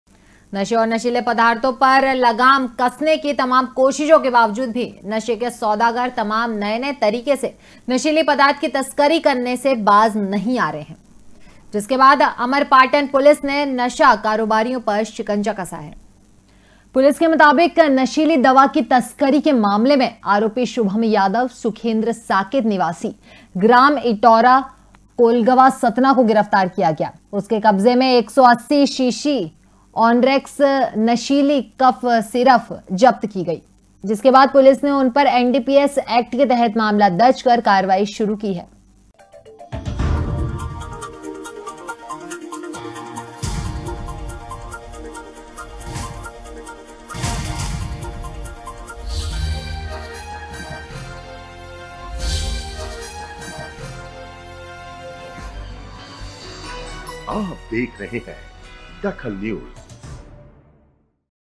DAKHAL NEWS अमरपाटन पुलिस की नशे के खिलाफ कार्रवाई
अमरपाटन पुलिस की  नशे के खिलाफ कार्रवाई
नशीली कफ सिरप के आरोपी को किया गिरफ्तार